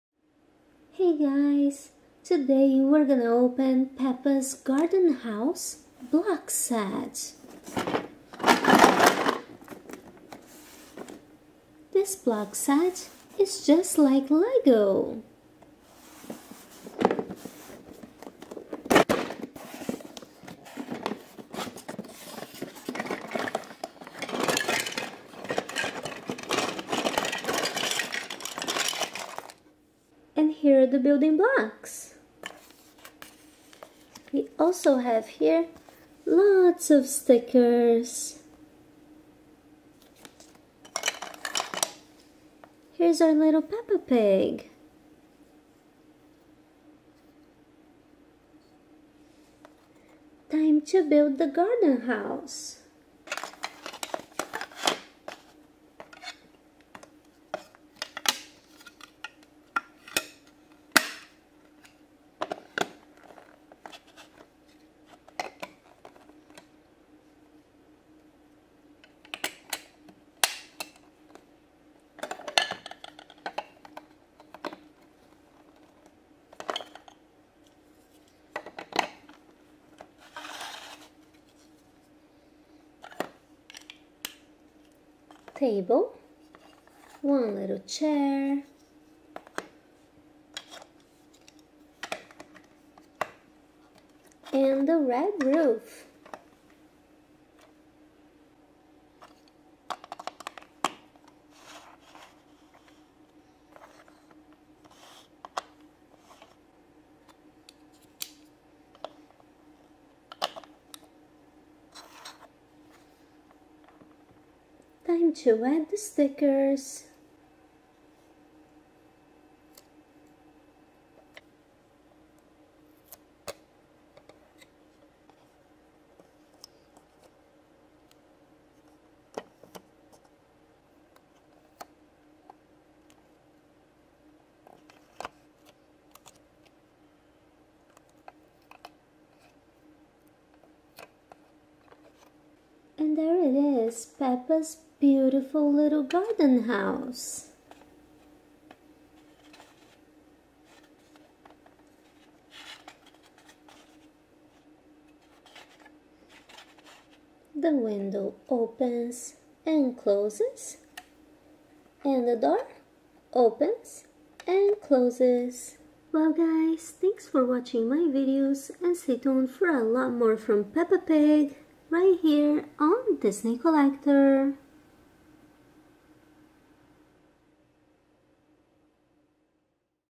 Peppa Pig Garden House Blocks Set Lego Duplo Compatible with Picnic Table by Disney Collector
Welcome to Funtoys Channel with surprise toys unboxing.